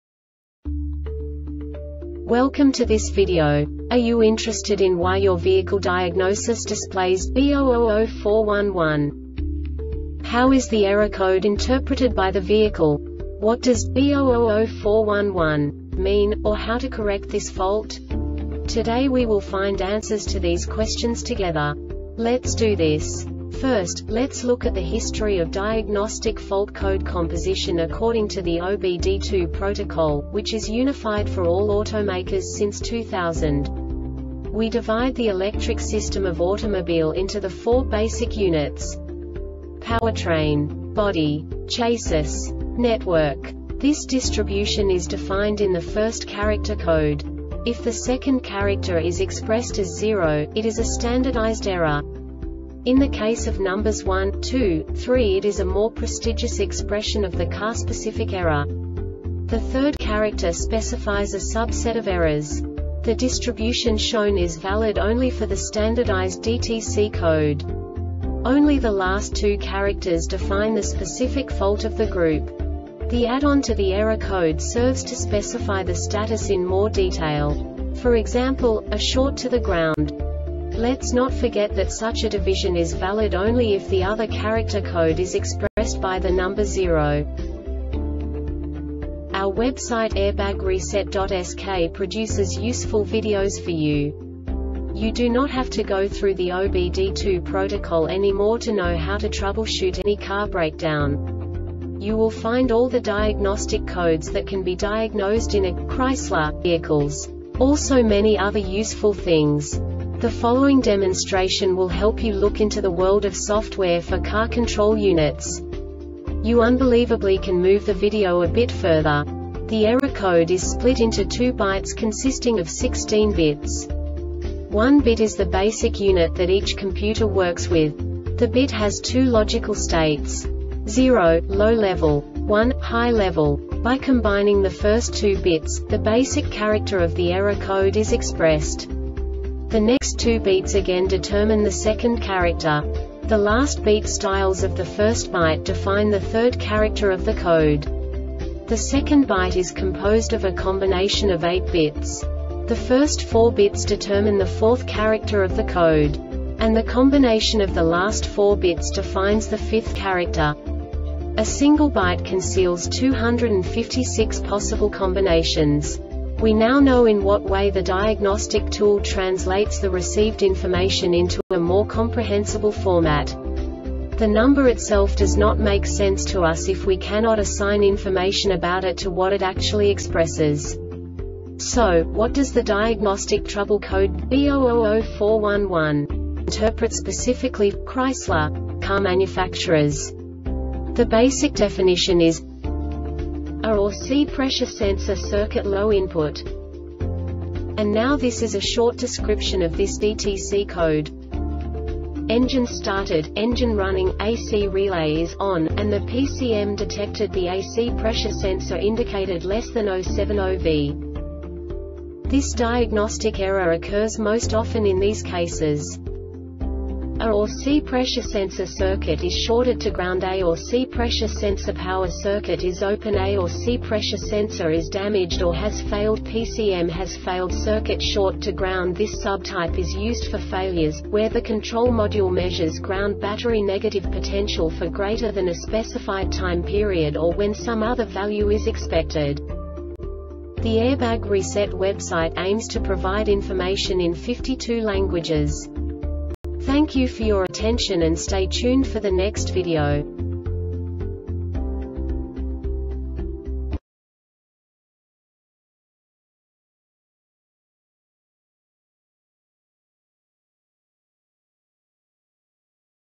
DTC Chrysler B0004-11 Short Explanation
Contents:
0:21 Basic DTC analysis according to OBD2 protocol standard.
1:48 Insight into programming
2:58 A diagnostically understandable description of the Diagnostic Trouble Code
3:05 Basic error definition

Make:
Chrysler
Code:
B0004 11
Definition:
A or C Pressure Sensor Circuit Low Input
Description:
Engine started,engine running,A or C Relay is "on",and the PCM detected the A or C Pressure Sensor indicated less than 0.70v.
Cause:
A or C pressure Sensor circuit is shorted to groundA or C pressure Sensor power circuit is openA or C pressure Sensor is damaged or has failedPCM has failed
Failure Type: